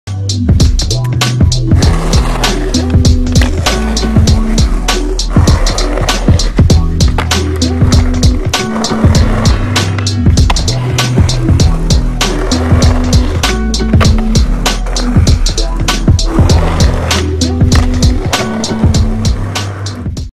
Snotnose Chris 04
Snotnose Skateboarding Rotterdam Chris Sørensen stadhuis-pyramid nollie frontsideflip inward heelflip nollie bigspin bigflip switch bigspin